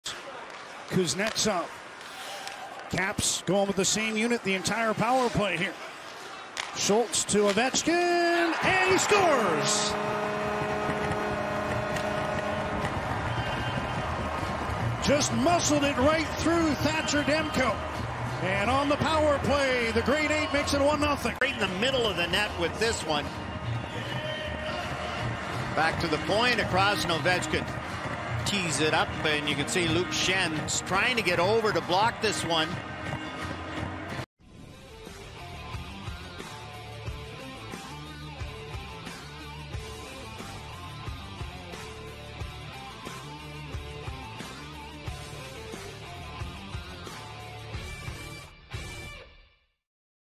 Alexander Ovechkin Powers The One-Timer Past Thatcher Demko For His 756th Career Goal
Watch as Alexander Ovechkin scores career goal number 756 by powering through the one-timer past Thatcher Demko and the Canucks.